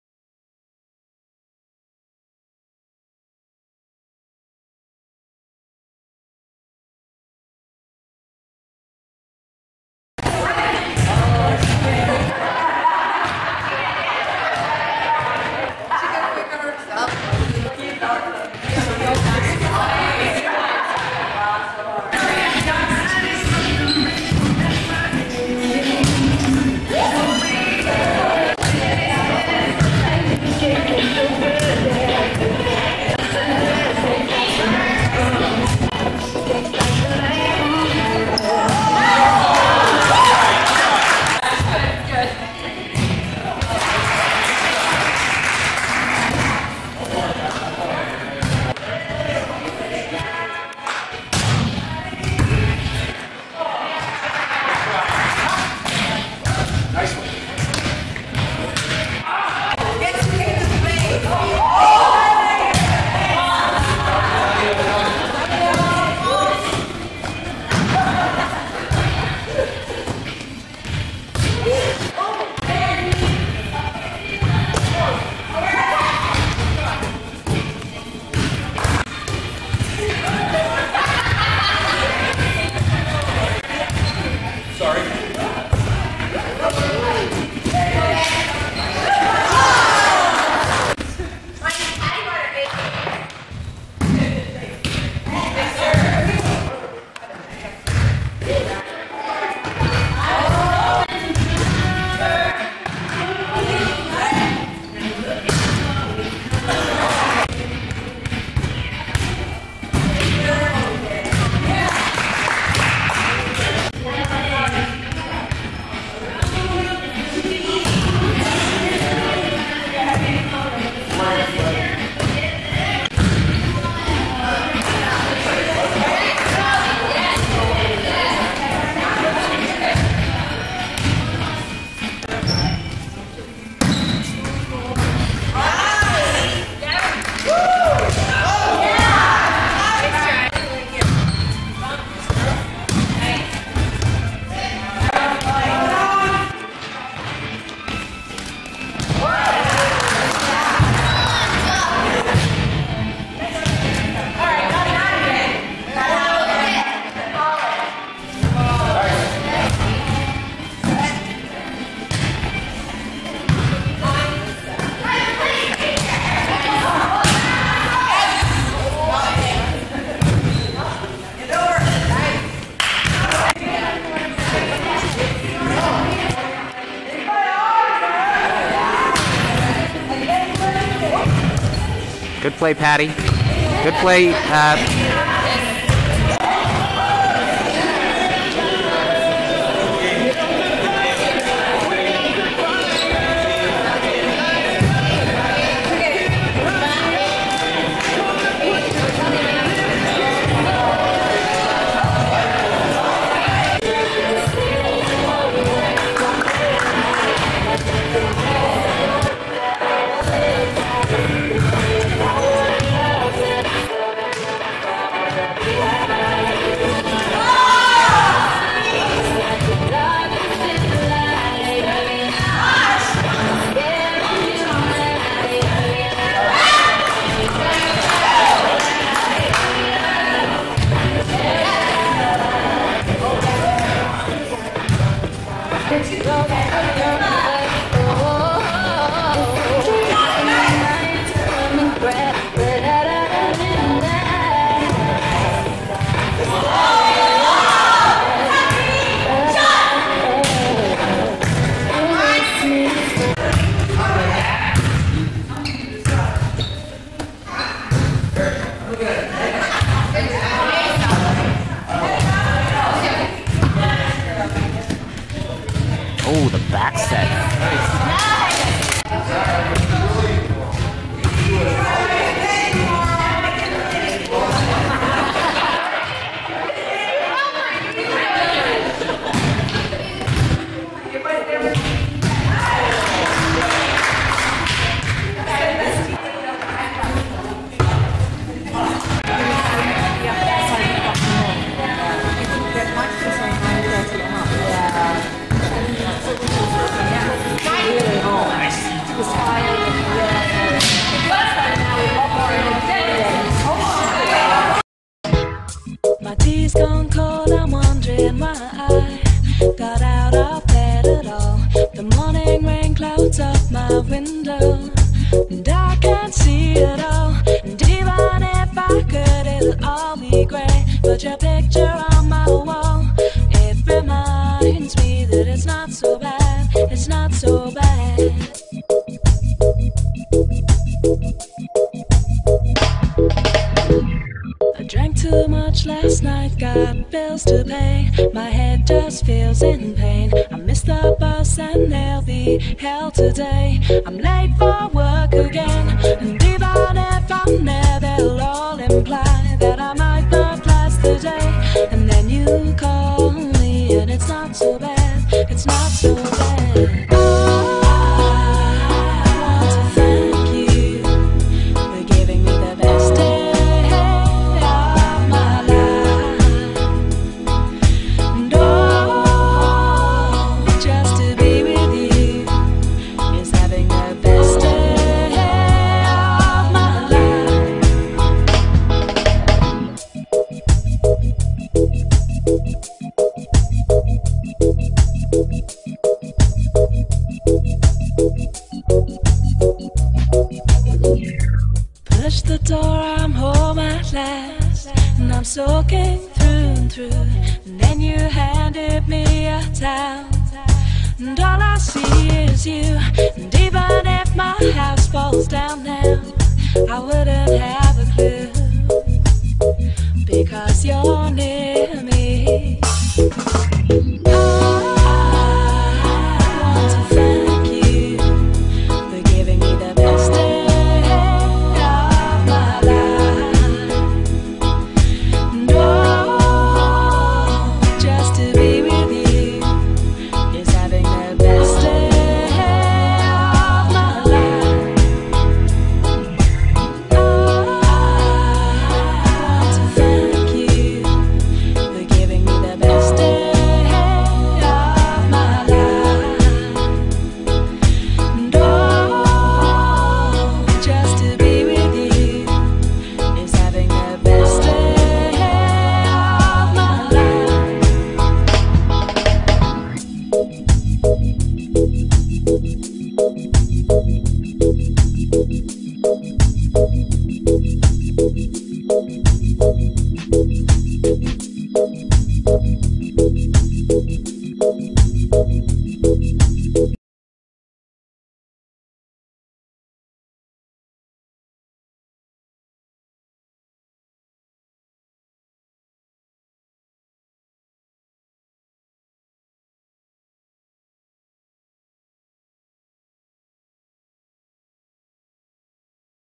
BRSC Mar 11 Game Action Final B Edited Version
This is an updated version of our BRSC Volleyball finals night (March 11)